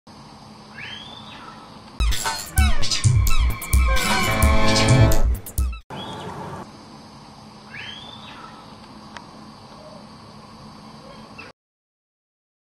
Who´s the boss here? Part 015
Shorts of the famous black swans of Ringenberg Castle.
Token raids, threats or actual attacks -
always funny to see how swans and people are interacting!

Swan attacks of the black swans at Ringenberg castle.

video: DokuNaut aka SupraNaut